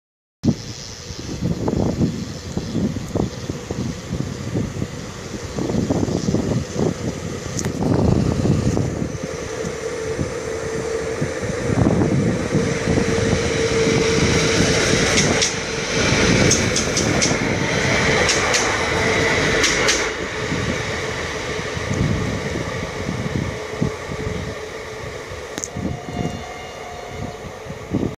Elron Stadler Flirt Dmu 2317 at Tartu(after crash)
August 2014.Tartu.2-3 tundi peale õnnetust